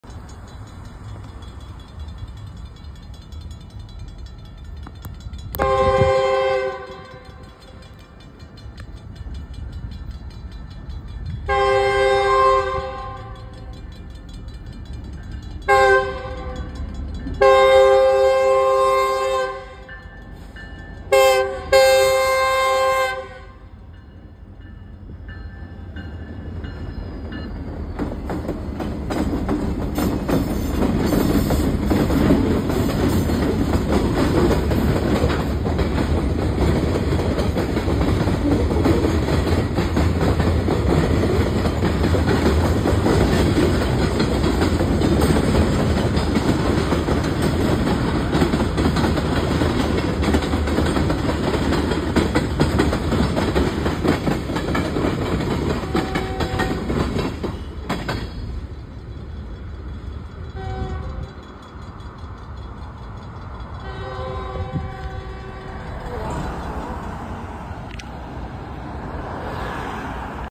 BNSF 3039 leads the second local back towards Watson Yard after passing Sepulveda Blvd
Taken just after 11:10pm with 3039 leading and 196 trailing and I got a video of them as they were heading towards Watson Yard from the Alcoa Yard with 10 cars 5 oil tanker cars and 5 hopper cars as they were heading towards Watson Yard and I was able to get it after hearing 3039s horn and enjoy the video everybody